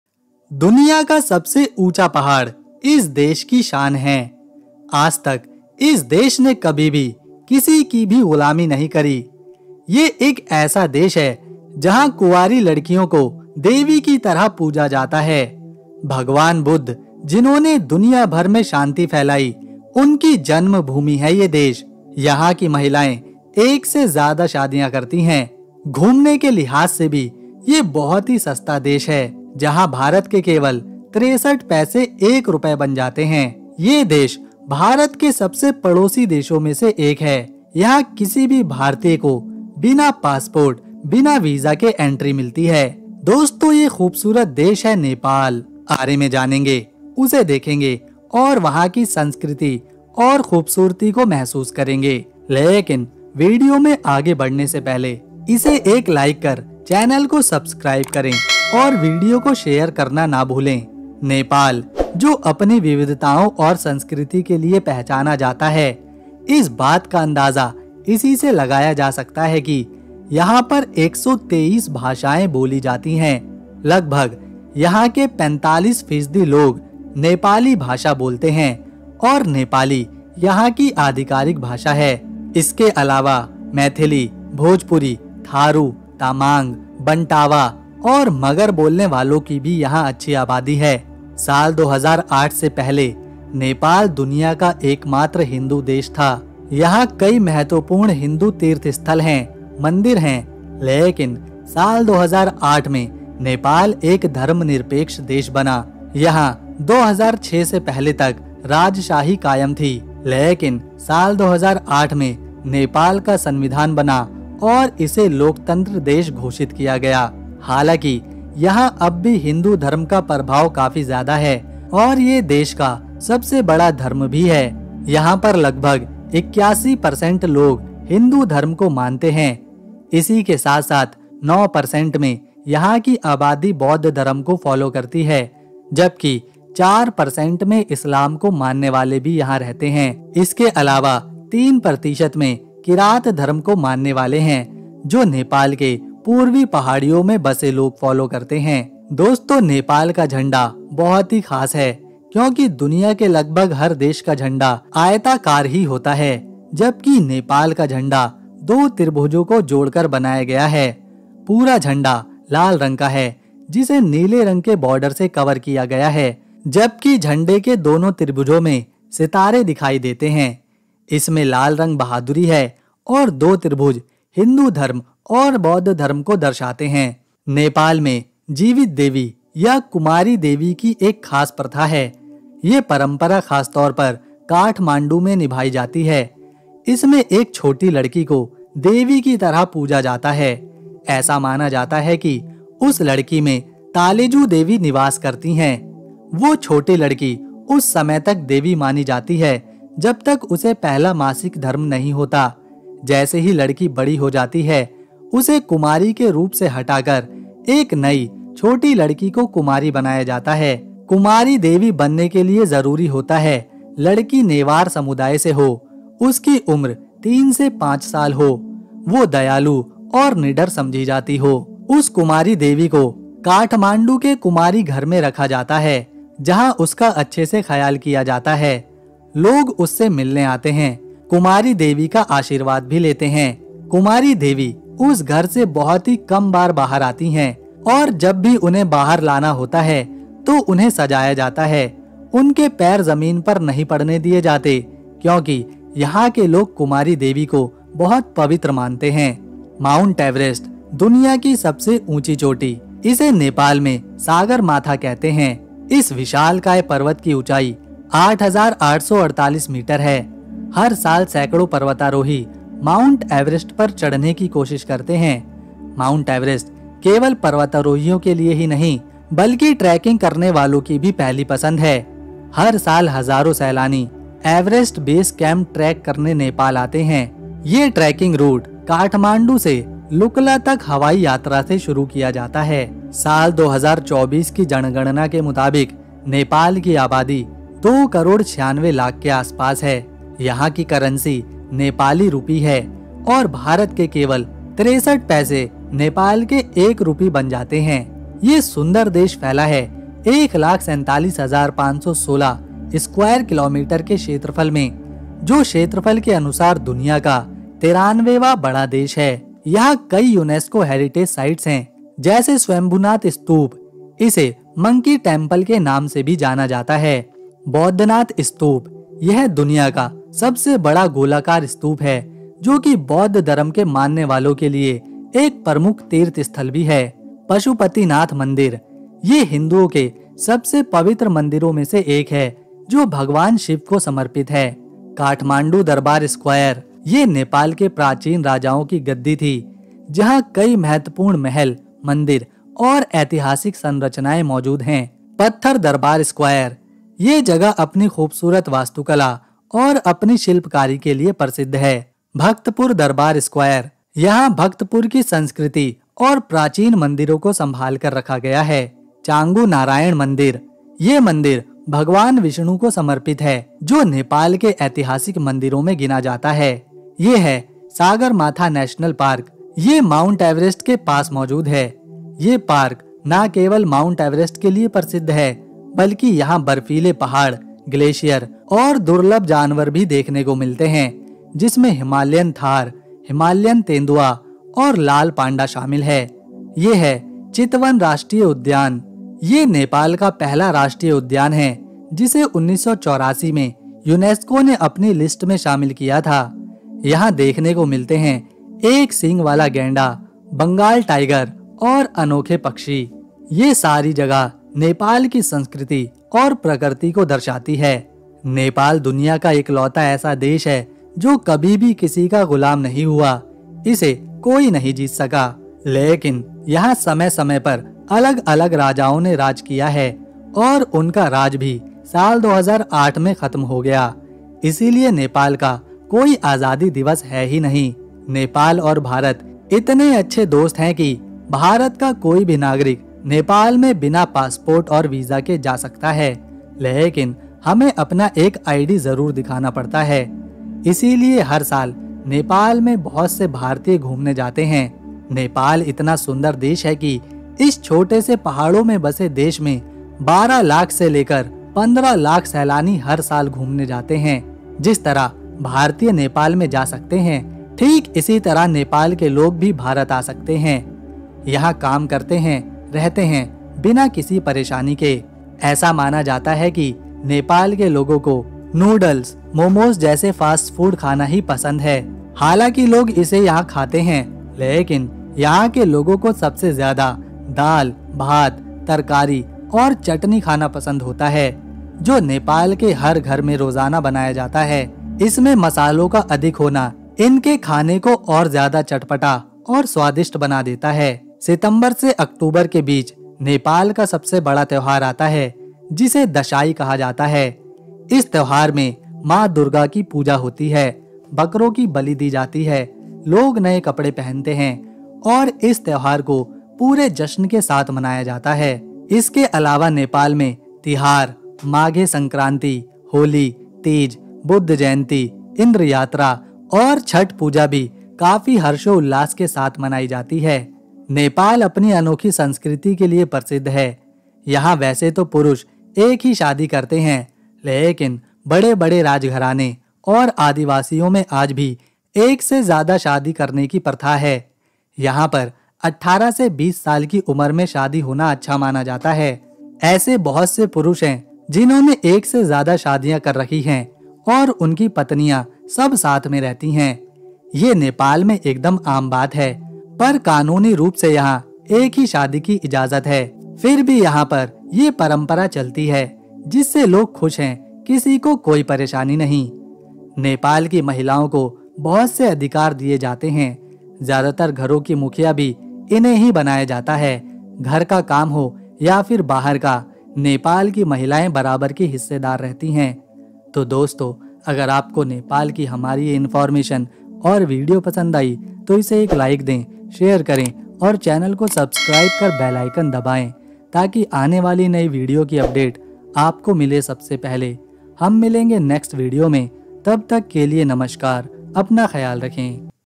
एक बार इस वीडियो को जरूर देखें | amazing Facts about Nepal Country travel and best places to visit.
Nepal, home to Mount Everest and rich in cultural heritage, blends stunning natural beauty with deep spiritual traditions. Its unique festivals and the worship of the living goddess Kumari make it truly special.

touropediahindi